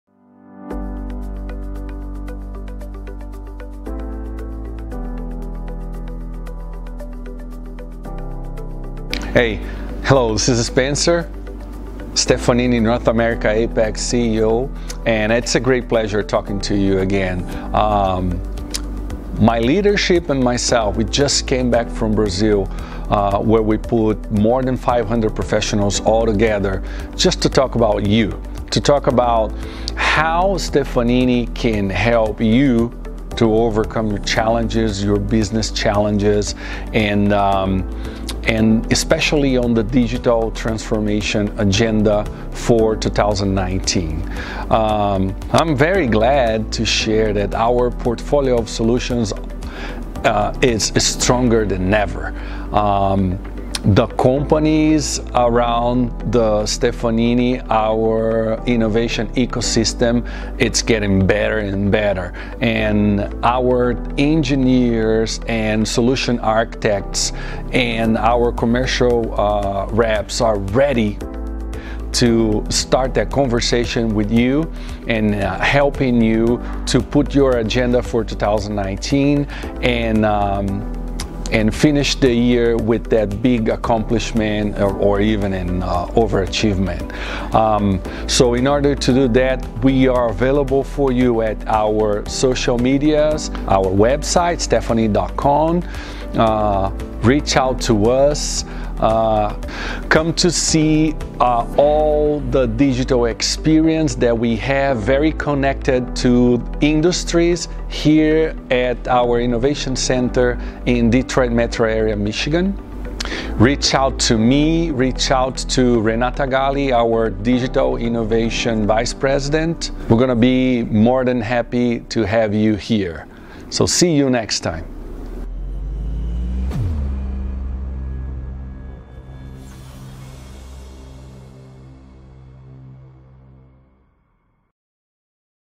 Perspective by Spencer Gracias - Kick off 2019
My leadership and I went to Brazil where more than 500 professionals gathered to talk about how Stefanini can help you overcome the business challenges you’re currently facing. At Stefanini, we co-create solutions for a better future, and we’re fully committed to helping you tackle your biggest challenges with digital transformation in 2019.